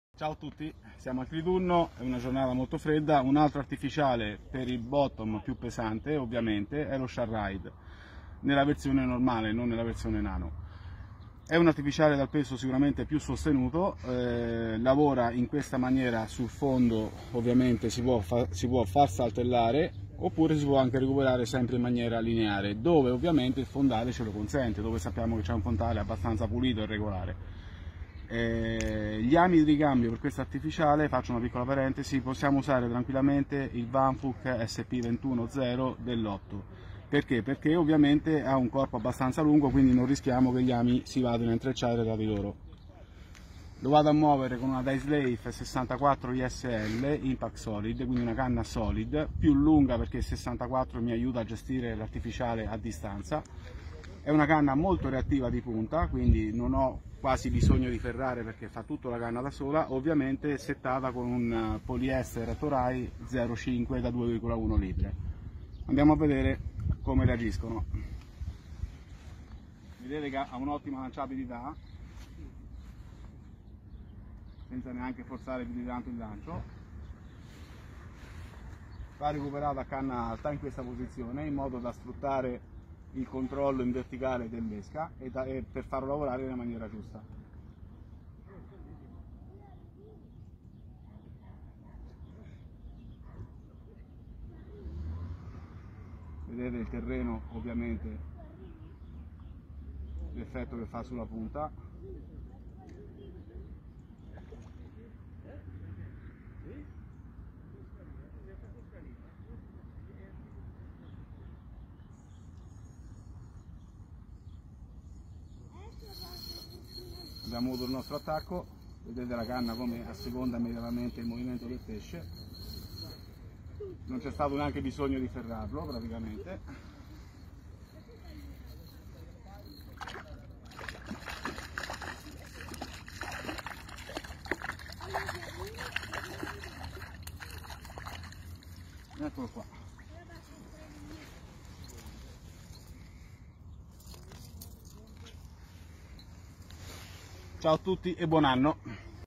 TROUT AREA ValkeIN Shine Ride prova in pesca
In questo video, realizzato dal T3 Specialist TROUT AREA Paolo Mancini, andiamo a scoprire il ValkeIN Shine Ride

ValkeIN Dainsleif 6,4 ISL Impact Solid
Toray Polyester 0,5 2,1 lb
Vanfook Sp-21zero#8
Della serie Zero Verge le canne indicate per questa azione di pesca sono:
 - 6'0 ISS Solid con Polyester Toray 0,5 2,1 lb
 - 6'3 XxxGL con trecciato PE Toray Real Fighter        0,025 (massimo 4/5 lb) oppure Toray Polyester 0,5 2,1 lb.
Setup ideale per il controllo di artificiali su laghi di dimensioni importanti.
Buon anno a tutti!!